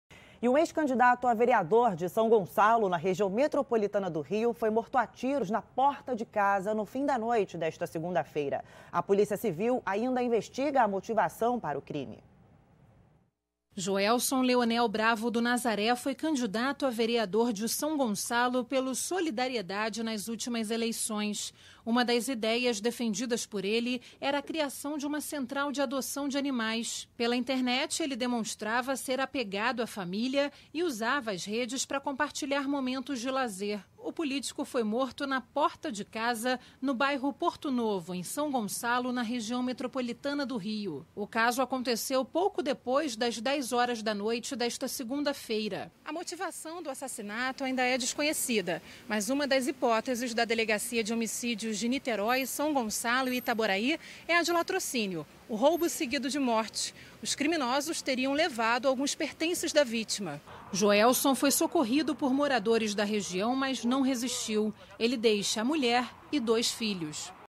Ex-candidato a vereador é morto em São Gonçalo
Um ex-candidato a vereador de São Gonçalo, na Região Metropolitana do Rio, foi morto a tiros, na porta de casa, no fim da noite desta segunda-feira. A polícia ainda investiga a motivação para o crime.